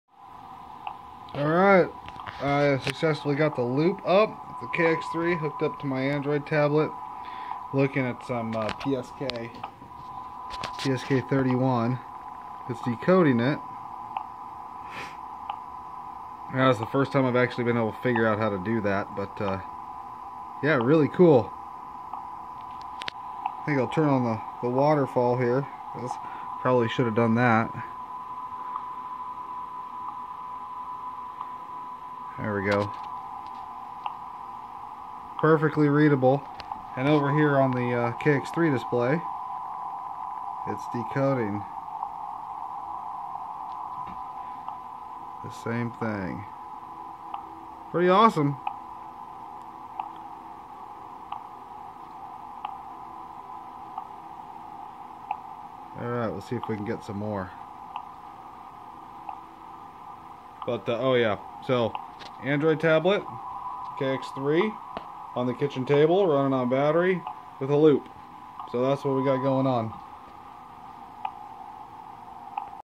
Elecraft KX3 decoding PSK31
I hooked up the KX3 to my android tablet to decode some PSK31.